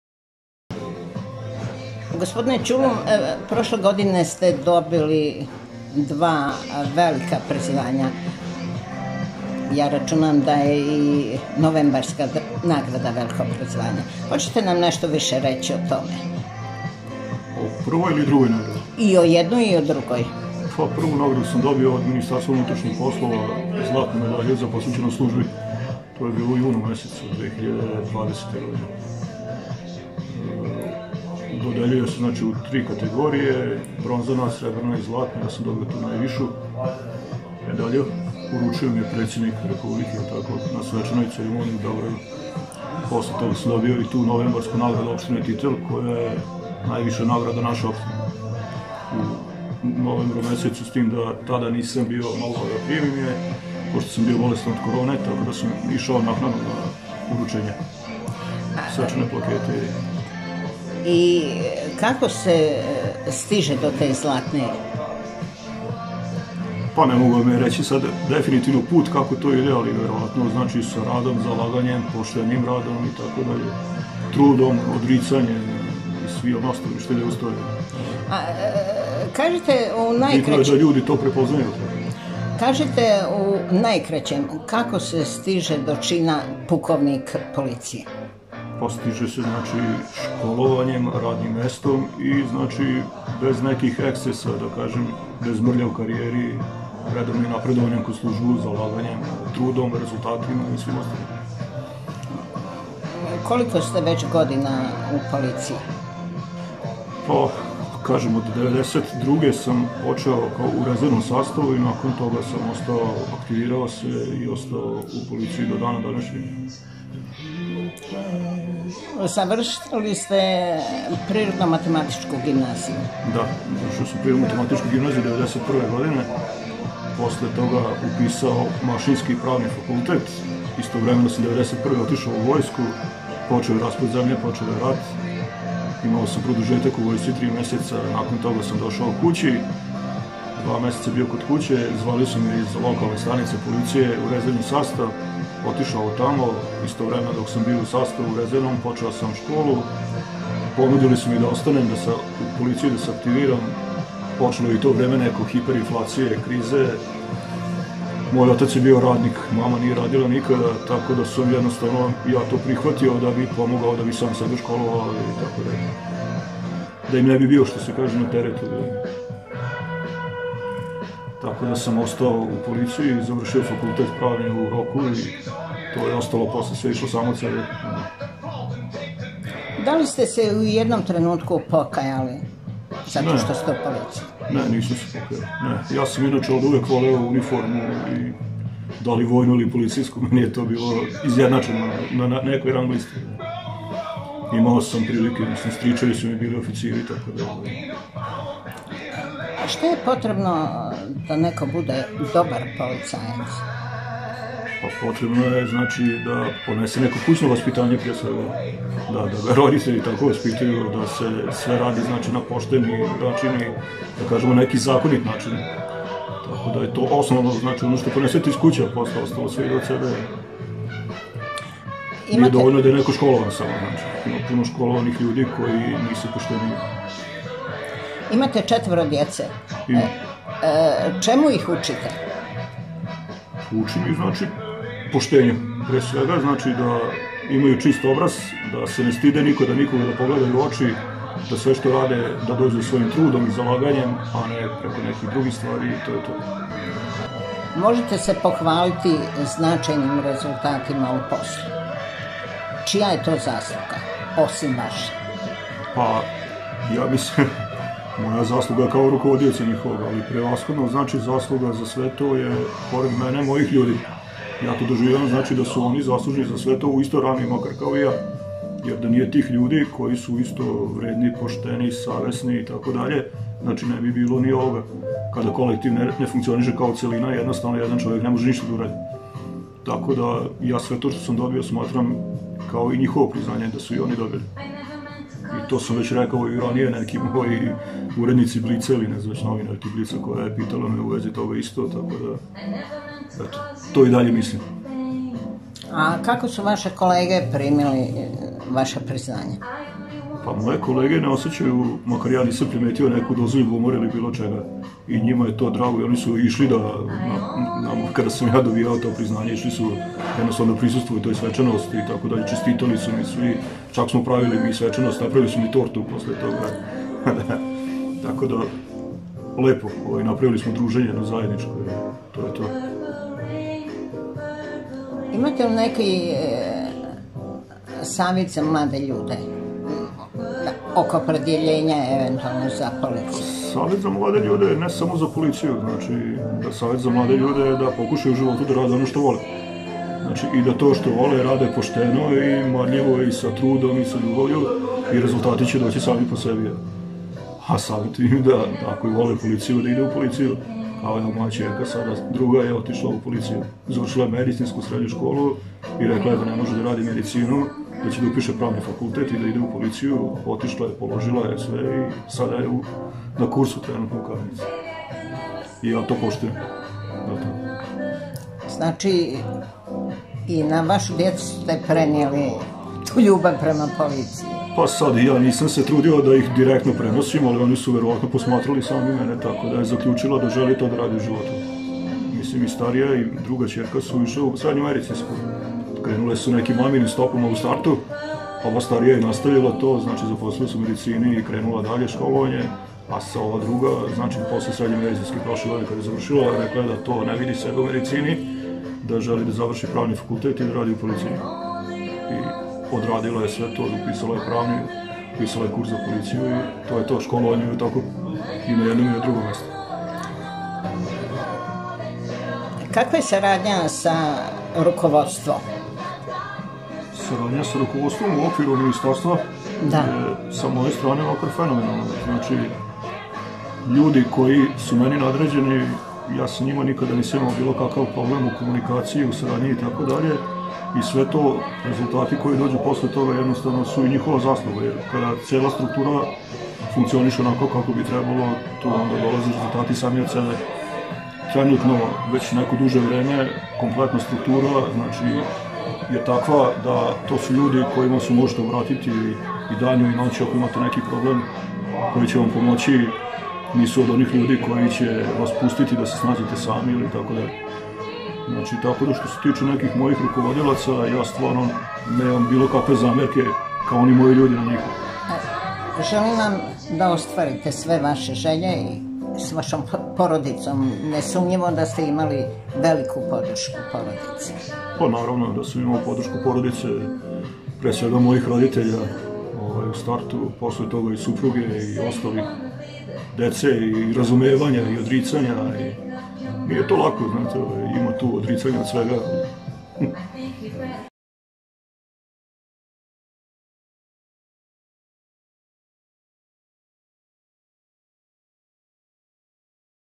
Milorad Ćulum, pukovnik policije, dobitnik je najvećih priznanja
Milorad Ćulum iz Šajkaša, pukovnik policije, zaposlen je u MUP-u kao šef odseka u sektoru unutrašnje kontrole u Novom Sadu. Njegova posvećenost službi doprinela je otkrivanju velikog broja krivičnih dela za šta je prošle godine nagrađen Zlatnom medaljom za hrabrost i posvećenost službi.